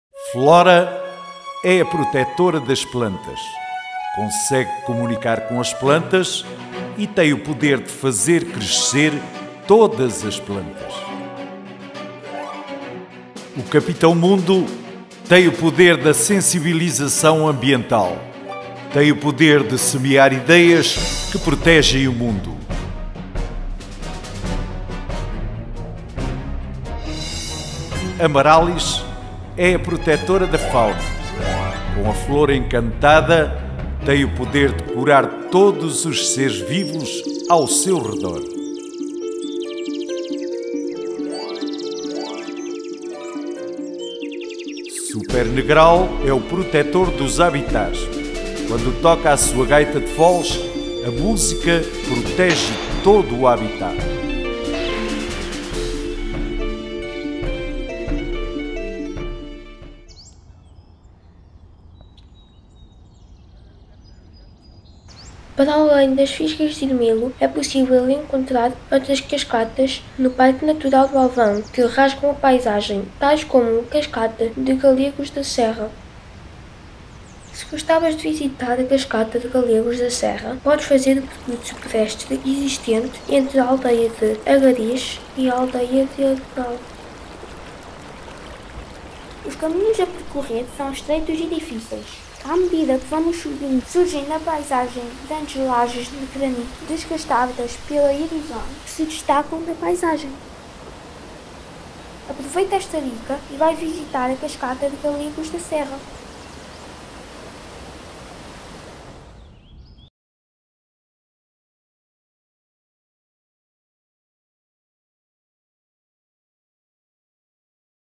FNT03E24 - PN do Alvão: Cascatas
Fruto da milenar interação do Homem sobre o território, exprime-se aqui um património etnográfico muito rico e diversificado que não se pode deixar de visitar.